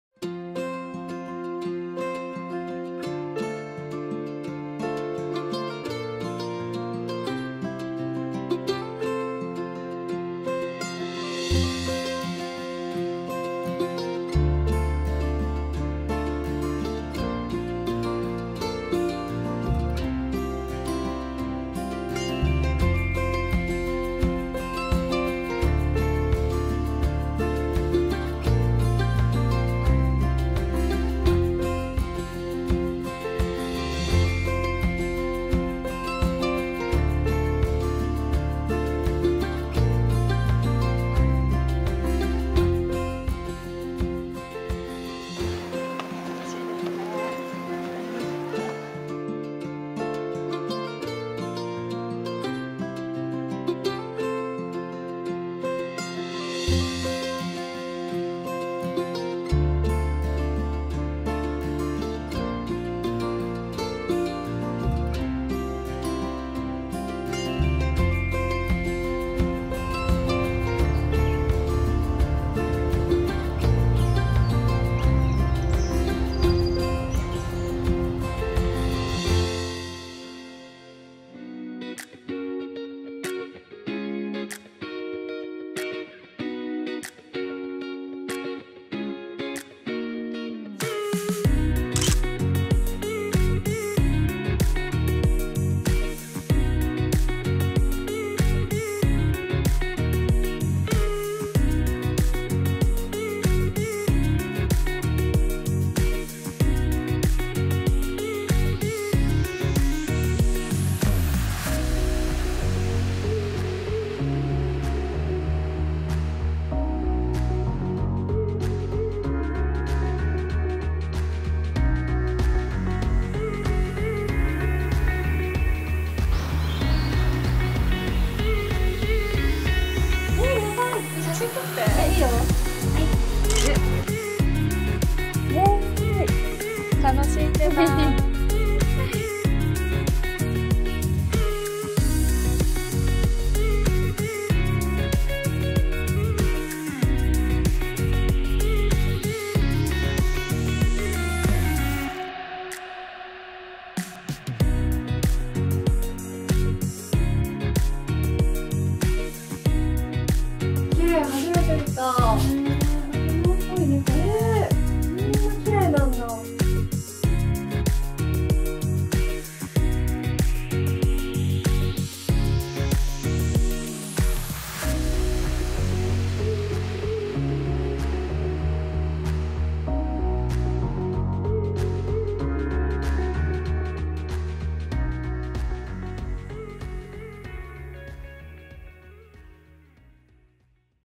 【琉球王国の足跡をたどる Special Movie】Re:fresh ～癒しの聖地を辿る、水から海へ～
Re:member URASOE～古に想いを馳せる新しき旅～
【Re:fresh コース　車とシェアサイクルで巡る一日コース】
沖縄県浦添市（うらそえし）で”琉球王国の歴史や浦添市の自然”を感じる癒しの旅を。徒歩で景観を楽しみながら、写真に残したくなるような街並みやグルメなどのおしゃれスポット巡り、自然を感じる体験ができるのがこの旅の魅力。
動画と共に、厳選したおすすめ女子旅コースをご紹介します！